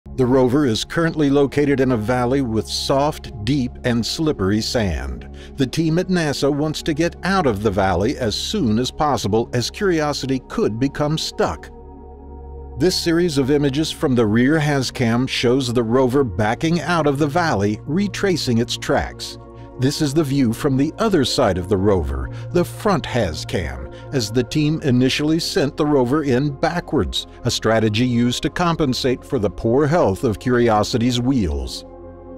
Mars rover escapes from 'Hidden Valley'
Curiosity escapes Hidden Valley by retracing its tracks. The soft sand in the valley was extremely slippery and almost caused the rover to become stuck.

Your contributions are appreciated so much and allow us to keep making videos about Mars.

Credit: NASA/JPL-Caltech/MSSS/Cornell Univ./Arizona State Univ./SSI